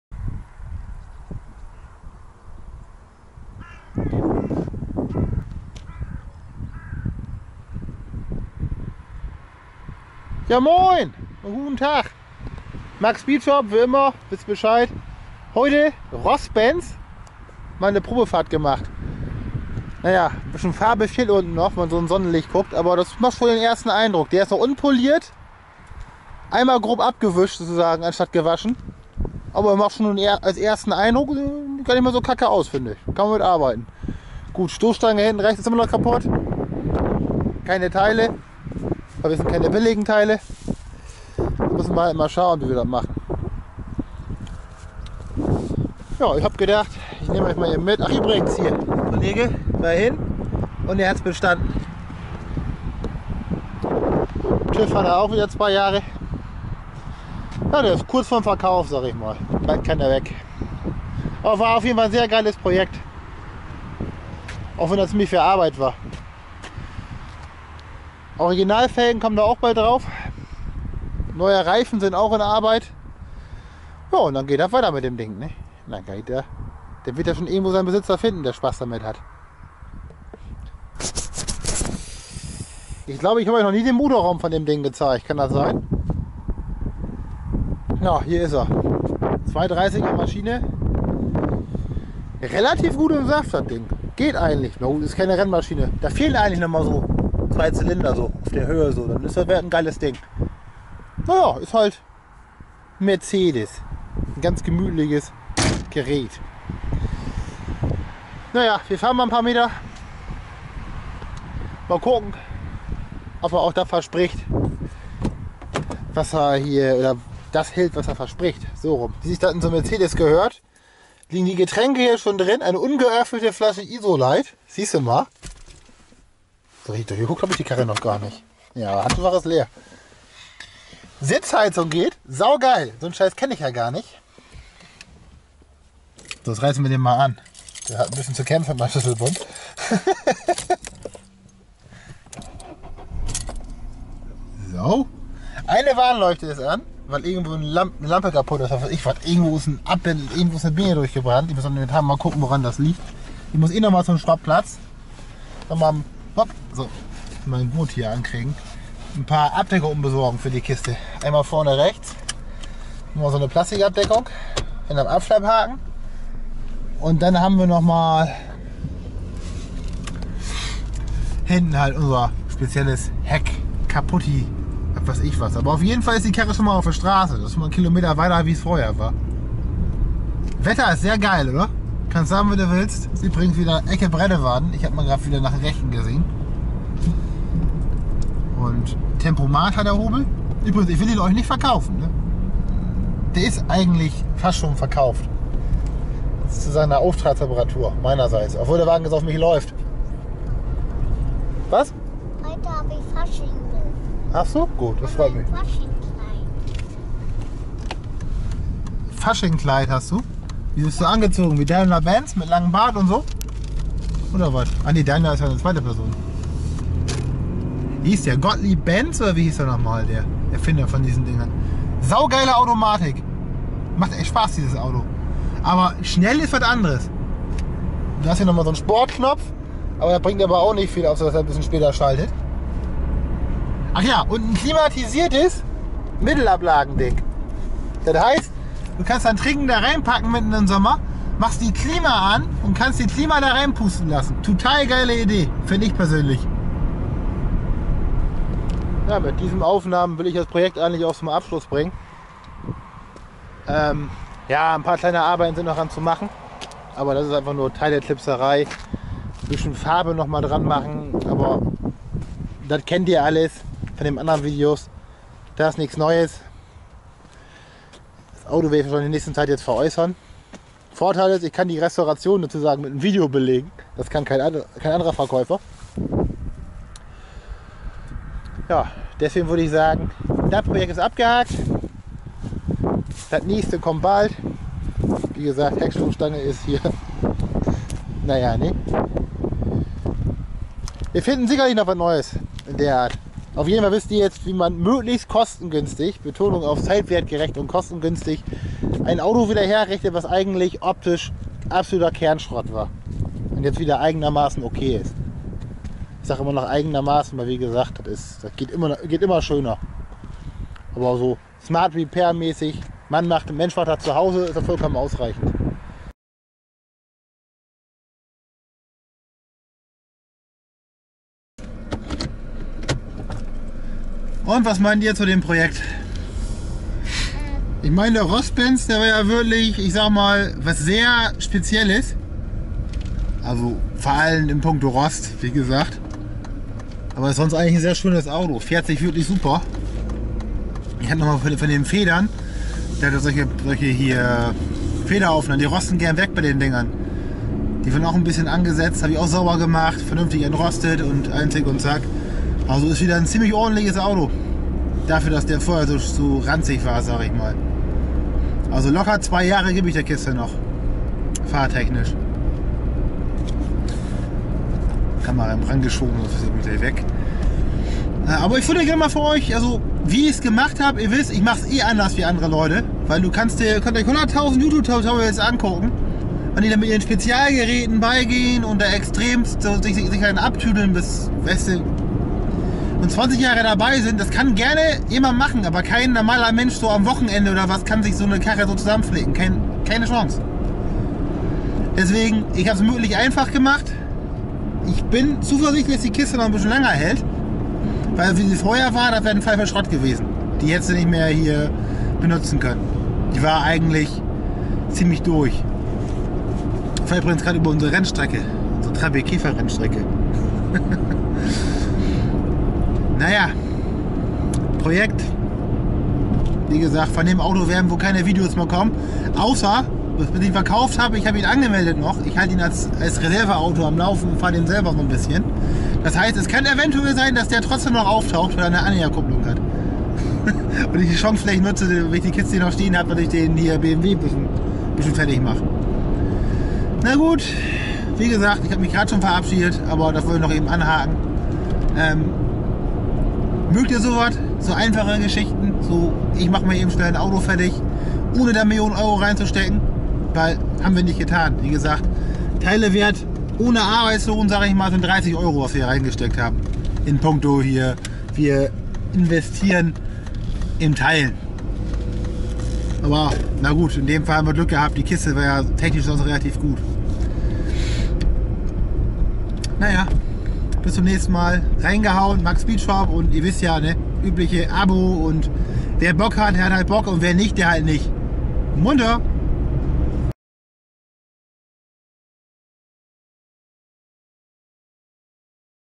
Mercedes W210 Rostentfernung Teil 6 - Das Ergebnis
Moin, jetzt ist das Benzprojekt so ziemlich am Ende. Noch ein bisschen kleinkram, der es aber nicht wert ist ein Video zu machen steht noch an. Aber das tut hier kaum zu Sache. :D
Naja eine Schönheit ist er wohl immernoch nicht geworden, aber wenigstens wieder gewappnet für die nächsten 2 Jahre.